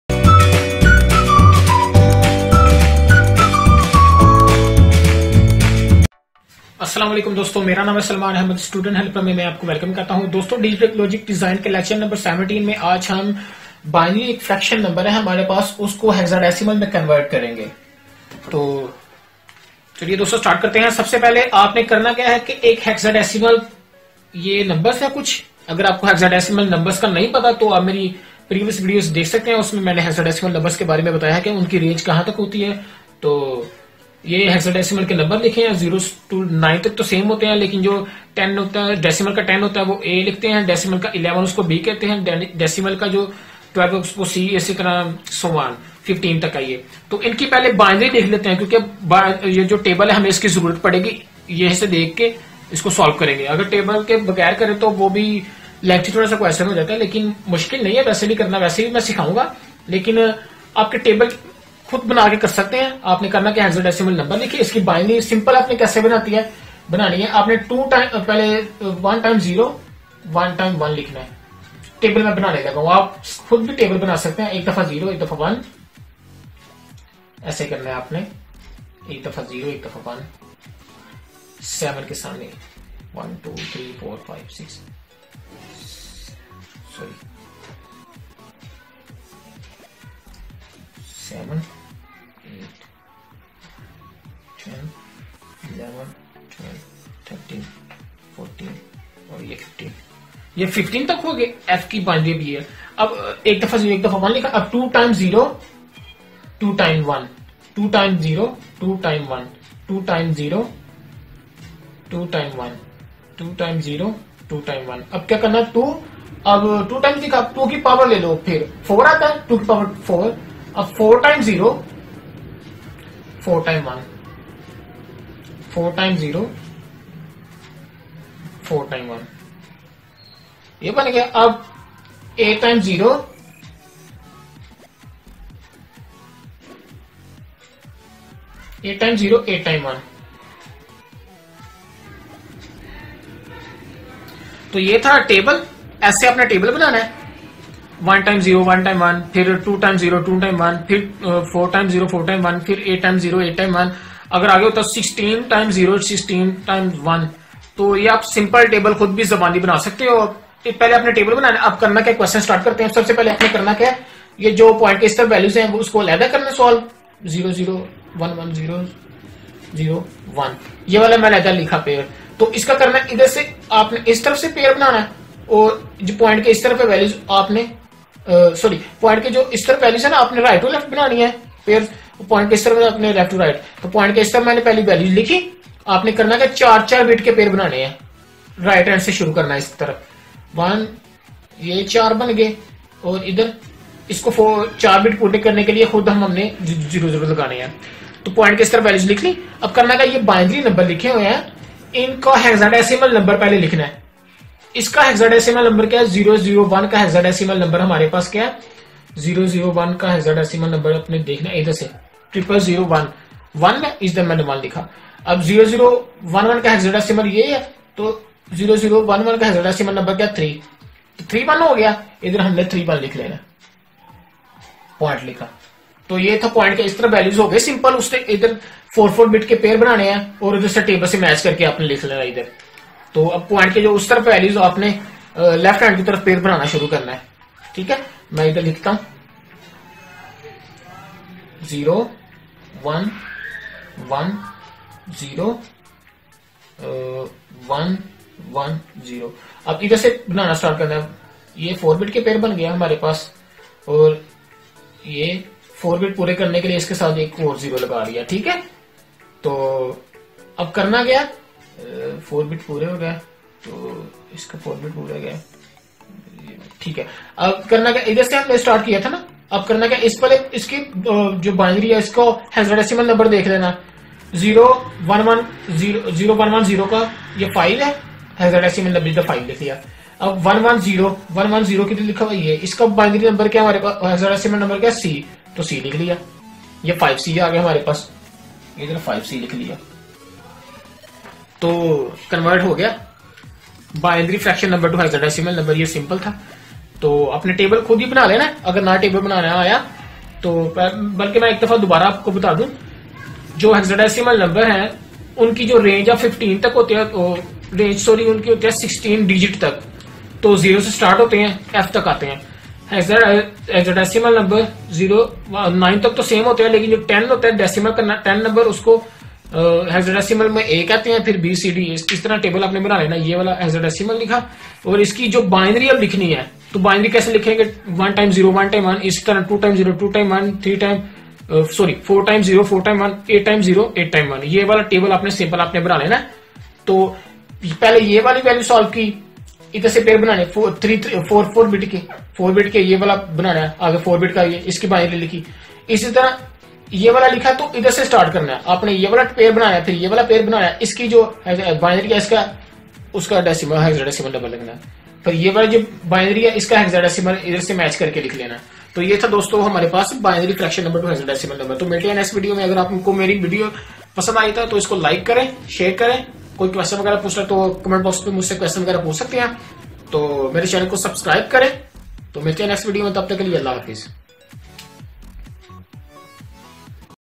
17.Binary Fraction to Hexadecimal |EASY EXAMPLES | DLD(URDU/HINDI)
Here You Can Learn Number System in very Easy and short method in URDU and HINDI , so if you want to get Good Marks in this course , then you have to watch this ...
I also Explain Number System Conversion in this Course ..
You Can Watch My All Videos on My channel  ...